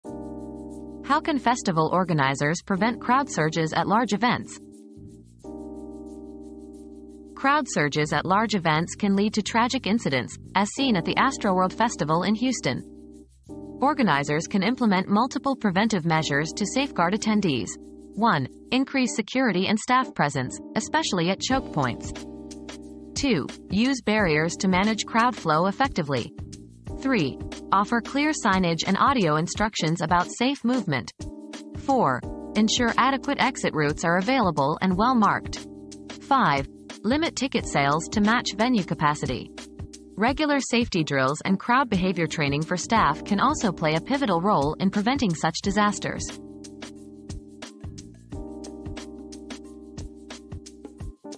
How can festival organizers prevent crowd surges at large events?
Preventing Crowd Surges: Essential Measures for Festival Organizers • Preventing Crowd Surges • Discover the key preventive measures festival organizers must take to avoid crowd surges and ensure the safety of attendees. From increased security to effective crowd flow management, learn how to create a secure and enjoyable event experience.